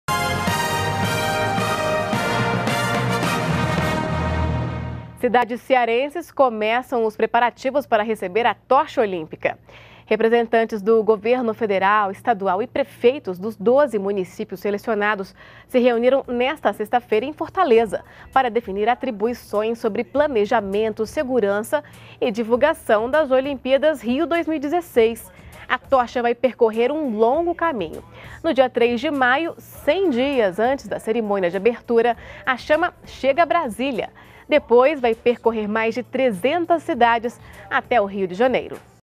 Cidades cearenses se preparam para receber a tocha olímpica
NBR NOTÍCIAS – 16.10.15: No Ceará, representantes do governo federal, estadual e prefeitos dos 12 municípios selecionados se reuniram, nesta sexta-feira (16), para definir atribuições sobre planejamento, segurança e divulgação das olimpíadas Rio 2016. A tocha vai percorrer 300 cidades de todas as regiões brasileiras. No dia 3 de maio, 100 dias antes da cerimônia de abertura, a chama chega à Brasília e após passar por todo o Brasil, chega ao Rio de Janeiro.